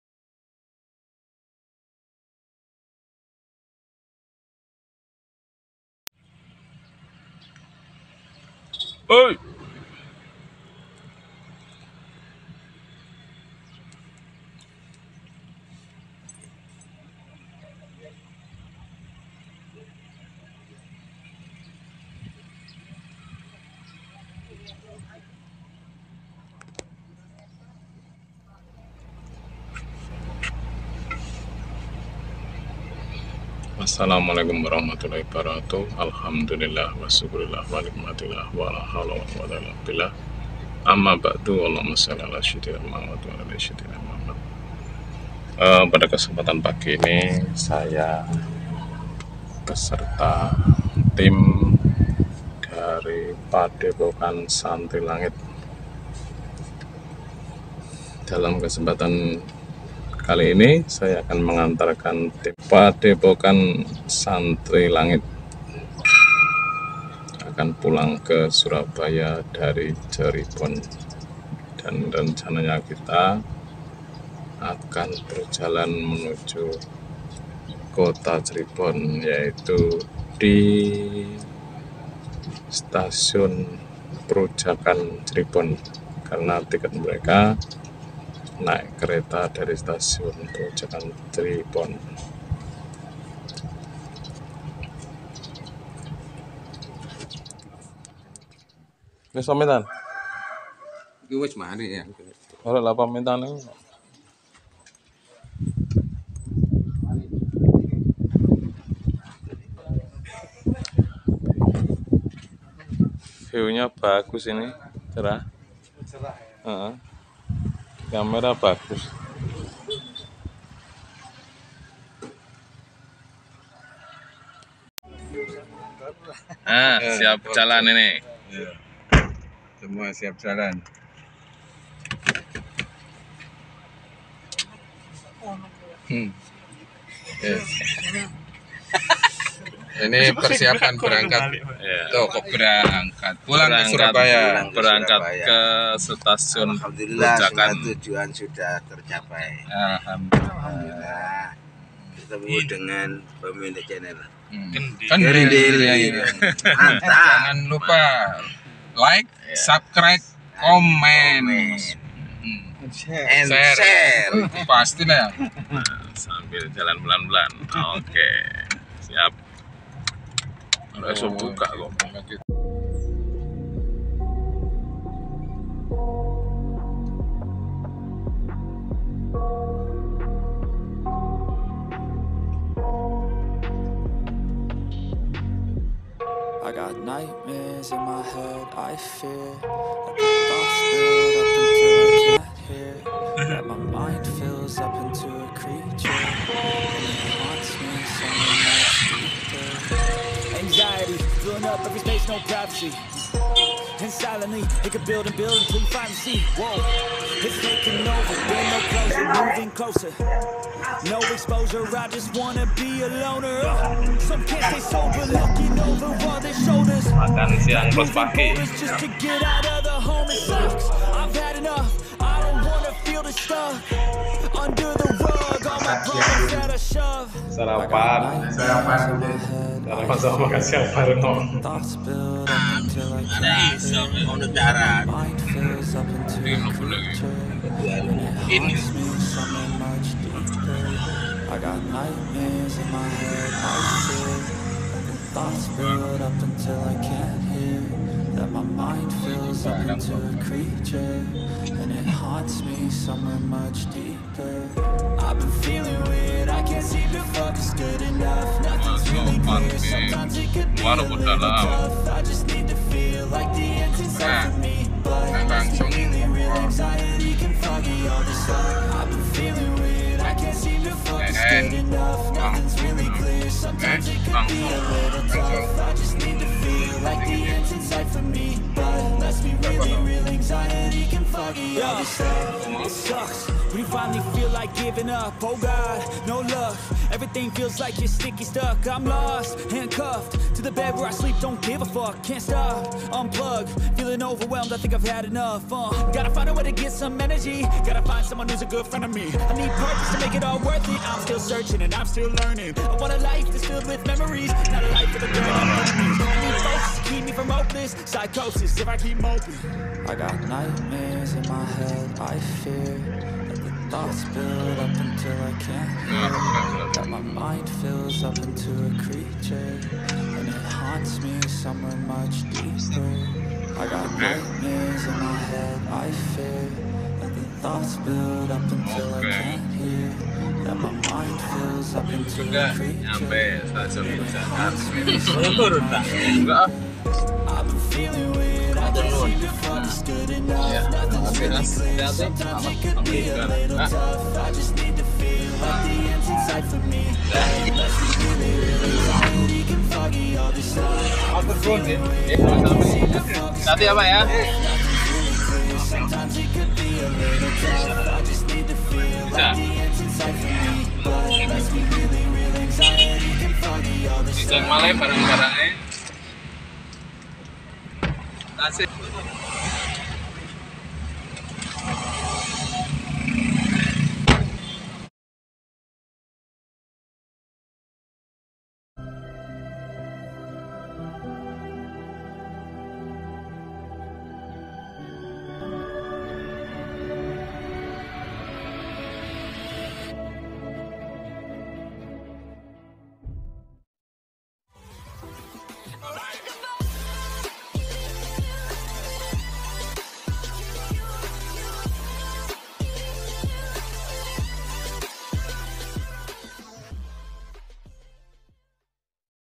Bersama Padepokan Santri Langit Surabaya
kenangan bersama padepokan santri langit Surabaya

kendil ireng mengantarkan tiem padepokan santri langit ke stasiun prujakan Cirebon untuk kembali ke Surabaya..semoga selamat sampai tujuan.